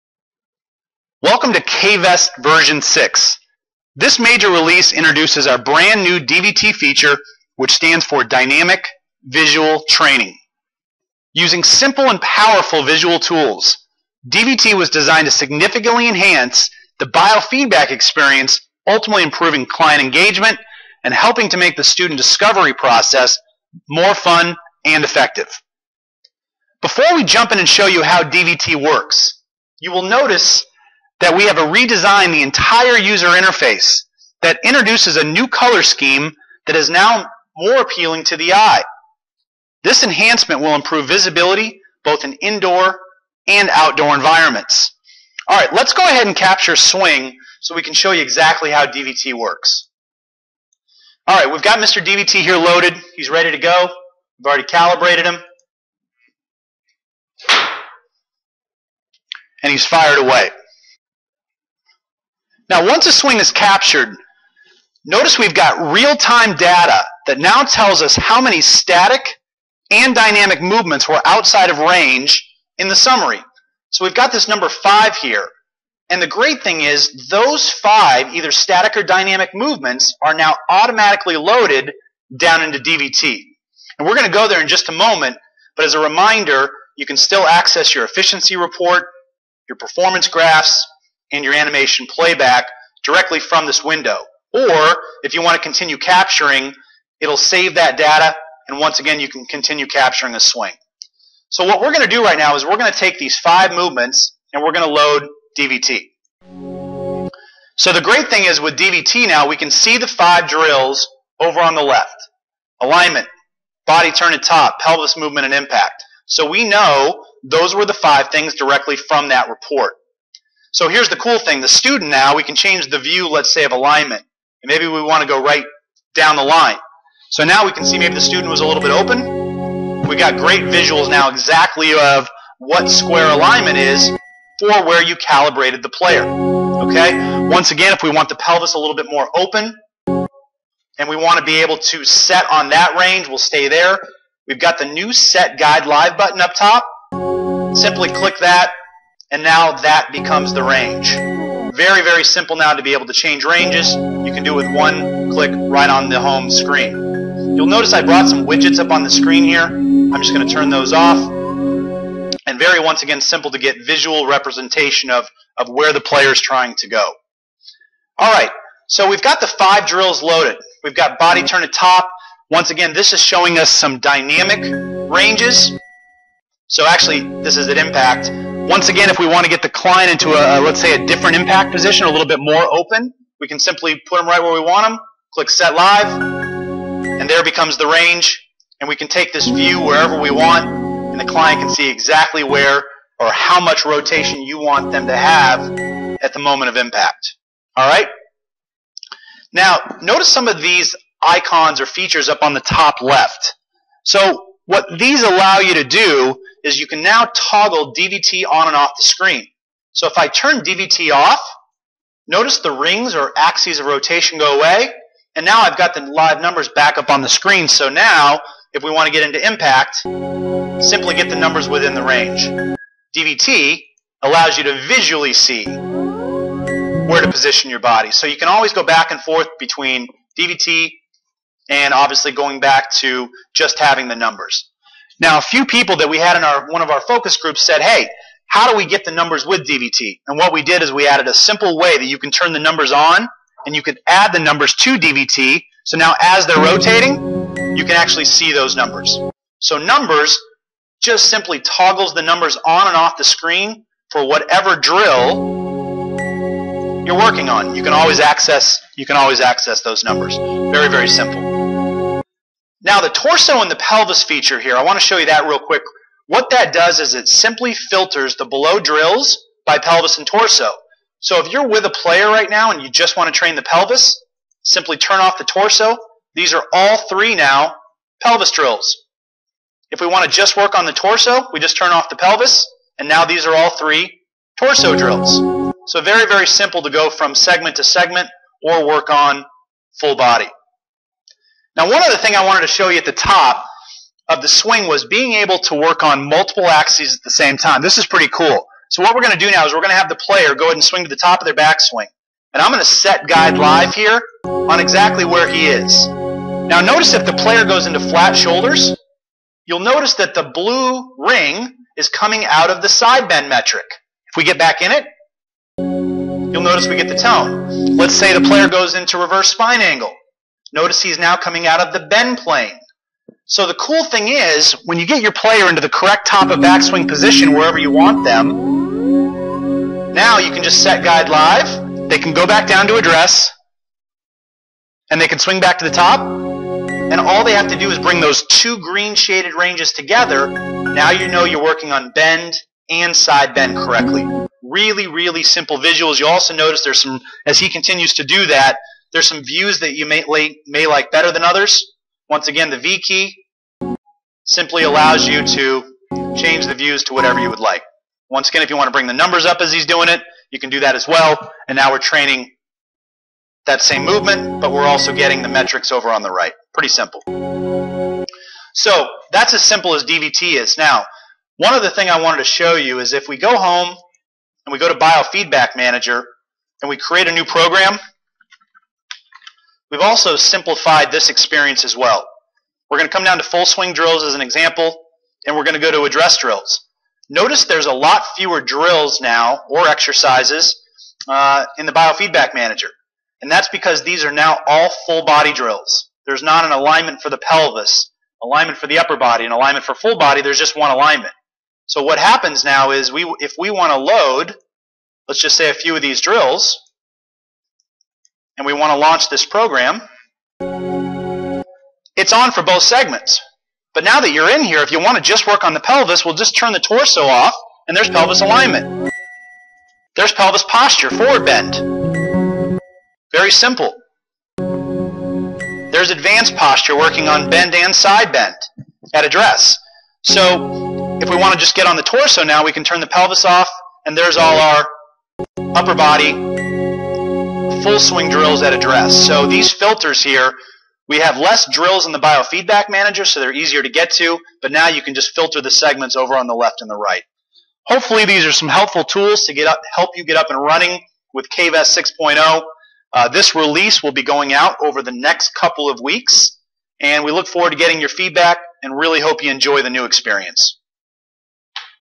K-VEST Unleashed v6 Release
This video shows some of the new functionality of K-VEST v6 including the new DVT (Dynamic Visual Training) feature. Schedule a K-Vest Demo Today!